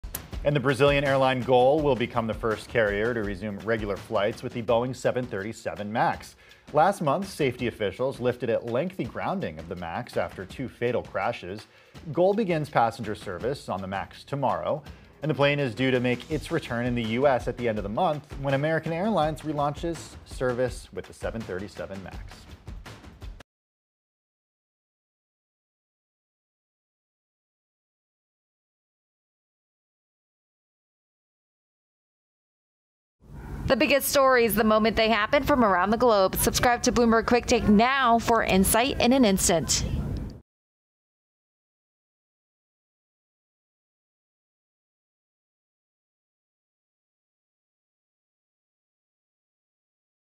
Boeing 737 Max Set to Resume Flying as Gol Plans Brazil Service
Brazil’s Gol Linhas Aereas Inteligentes SA said it will be the first airline to resume regular flights with Boeing Co.’s 737 Max since safety officials lifted a lengthy grounding of the embattled jet last month.

Passenger service on the Max will begin Dec. 9 on domestic routes from Sao Paulo, the airline said in a statement late Monday. Gol has trained 140 of its pilots in the U.S., using simulator sessions and a new curriculum approved by the Federal Aviation Administration and ANAC, Brazil’s regulator, in the wake of two fatal Max crashes.

Boeing’s best-selling jetliner is due to make its return in the U.S. almost three weeks later when American Airlines Group Inc. relaunches Max service on flights connecting Miami with New York. Boeing is also closing in on another milestone, the first post-grounding delivery of a Max plane, with a handover to United Airlines Holdings Inc. likely this week.

The moves will enable the Chicago-based planemaker to crank up a critical commercial machine after racking up about $20 billion in costs from the Max accidents, which prompted the longest aircraft grounding in U.S. history and sparked federal investigations and scores of lawsuits. Boeing redesigned key flight-control software after the crashes, which killed 346 people.

While Boeing has warned that it faces another year of losses brought on by the Covid-19 pandemic, its shares have soared 65% since the end of October, buoyed by the Max’s return and vaccine progress. That was the biggest gain during the period on the Dow Jones Industrial Average.

While Gol didn’t specify which destinations the Max would serve from Sao Paulo, the airline’s website said flights to Porto Alegre, Curitiba and Rio de Janeiro are among those that may be operated by the model.